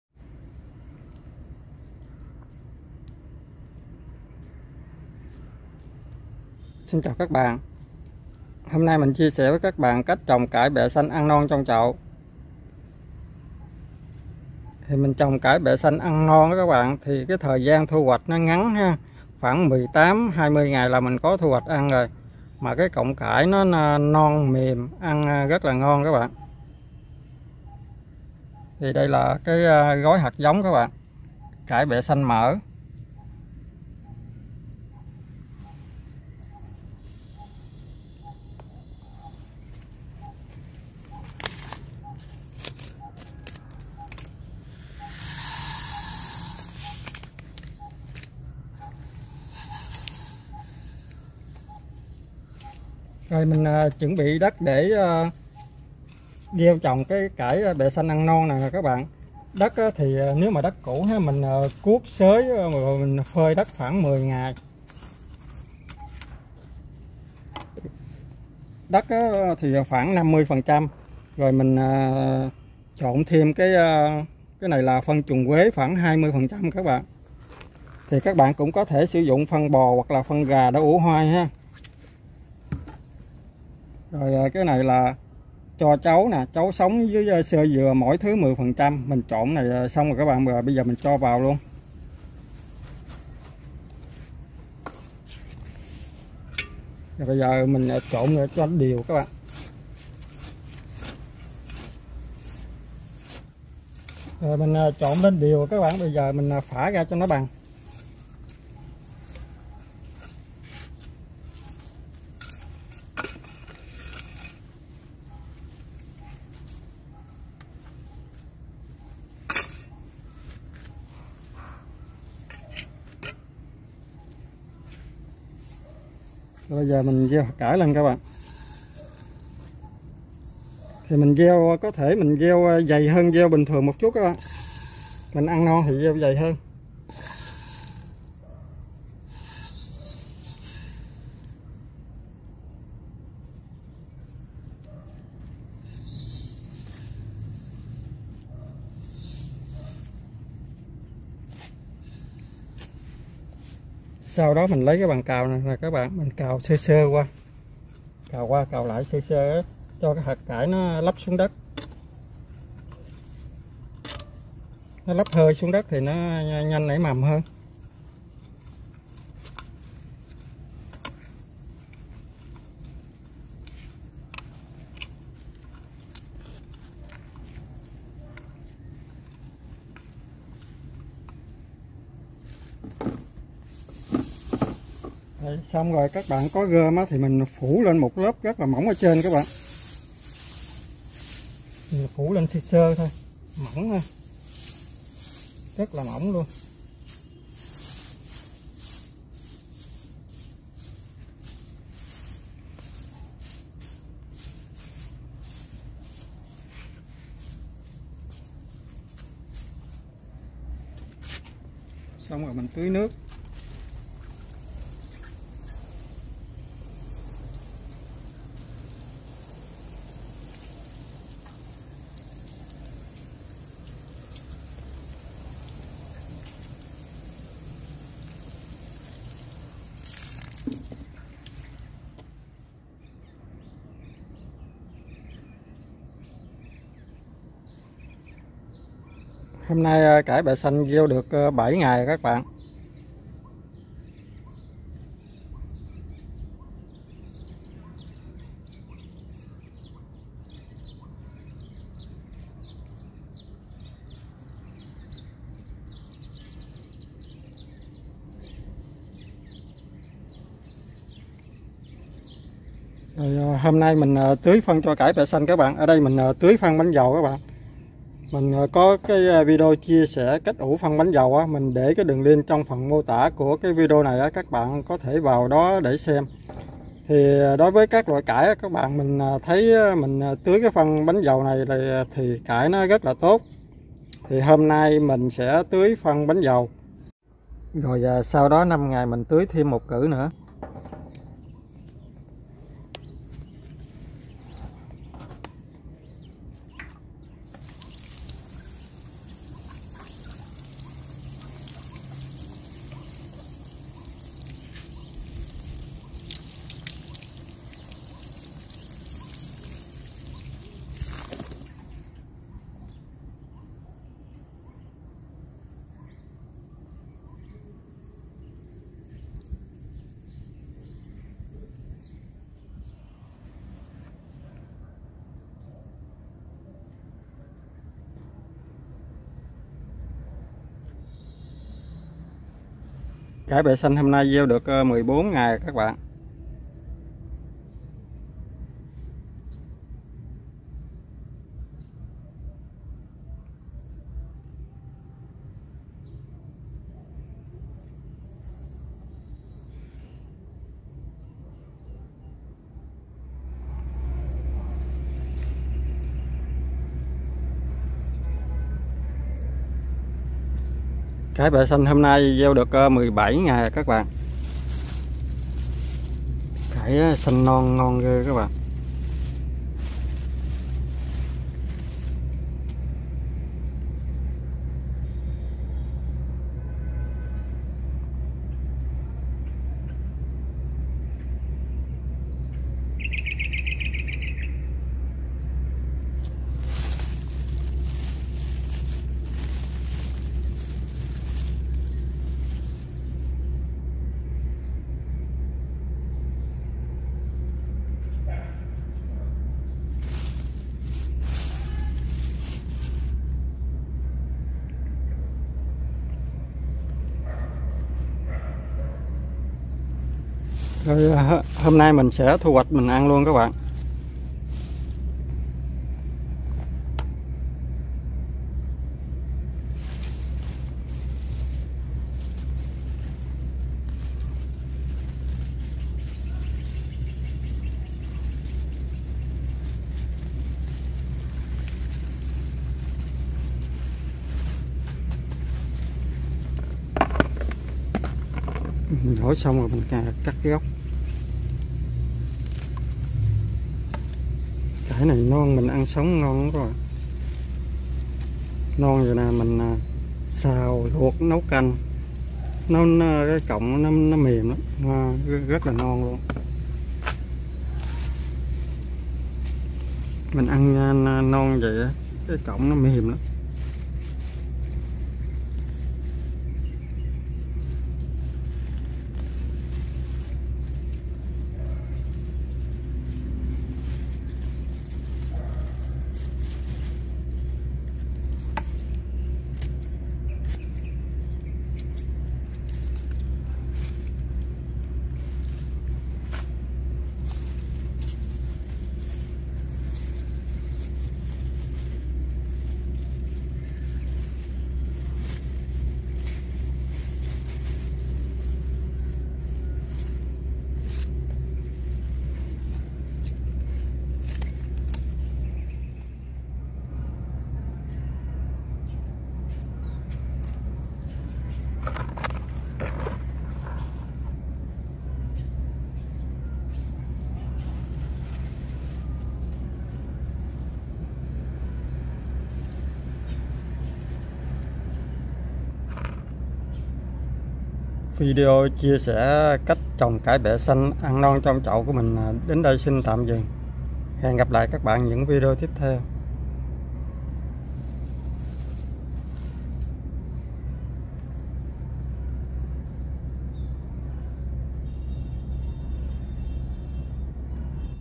Cách trồng cải bẹ xanh ăn non trong chậu | Phan Đức #200
Hôm nay mình chia sẻ với các bạn cách trồng cải bẹ xanh ăn non trong chậu. Cám ơn các bạn đã xem video của mình.